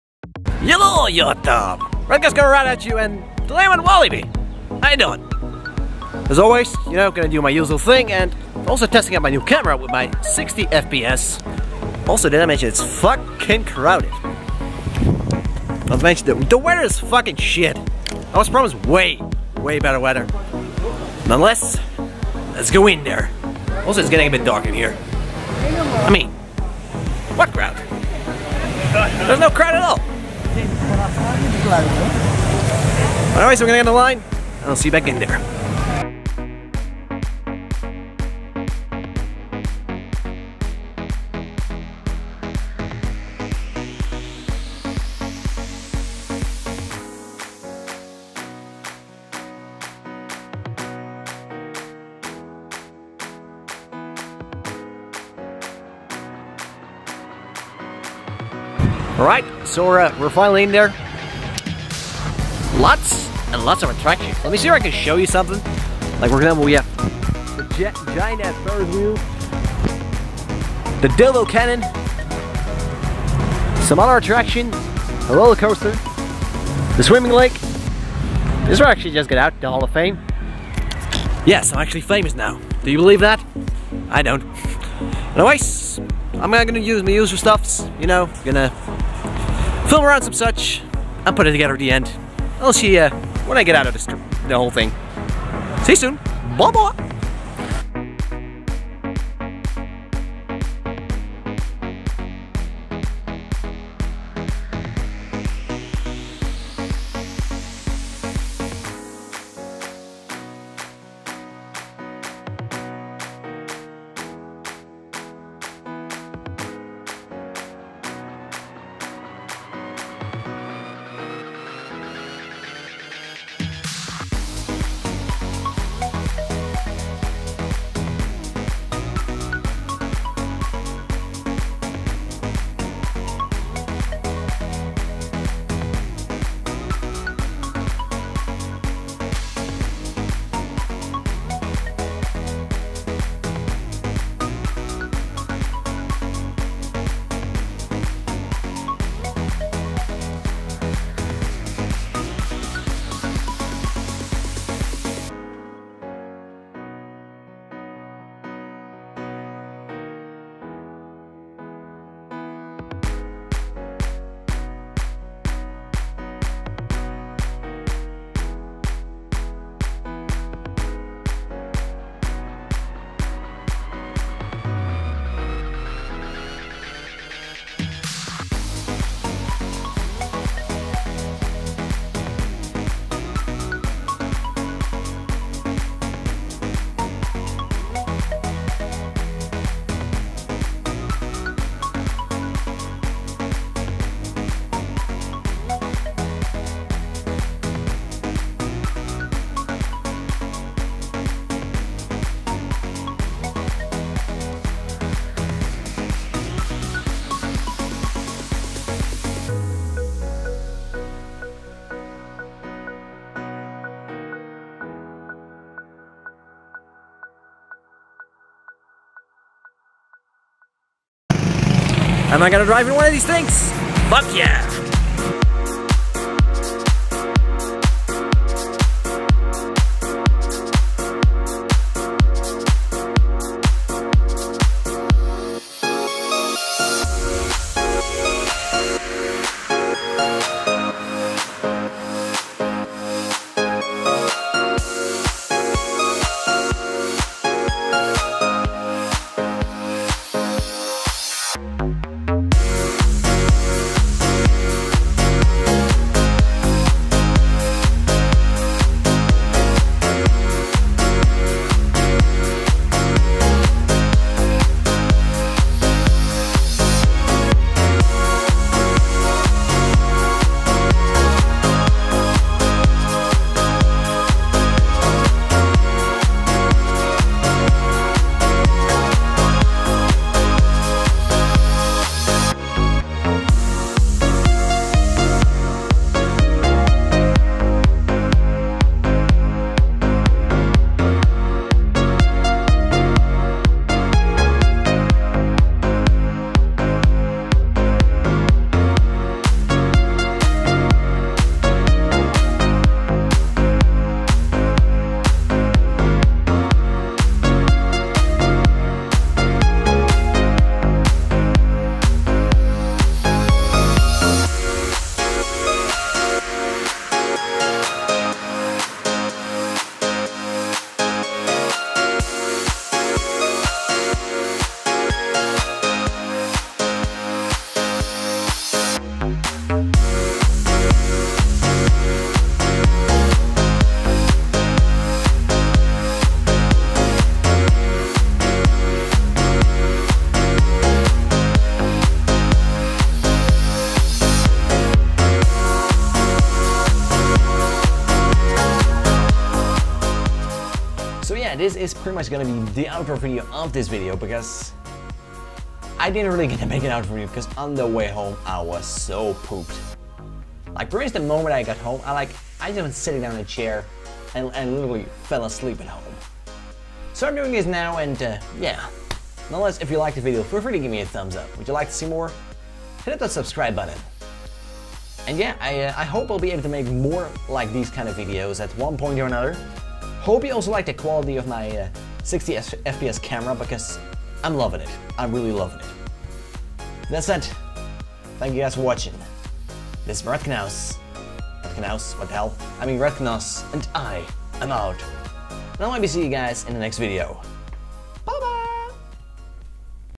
V-Logs - Walibi Holland 2016
Alrighty, attempted a sort of vlog! which seemed to worked out better then my vlog attempt @ First Look 2016, Improvement? perhaps!

Maybe it'll get easier when I'd do it more!

None the less, had a fun day at walibi, hope you enjoy!

Both from Epidemic Sounds.

For social stuffs go to my twitter!

And for Live Gaming Content!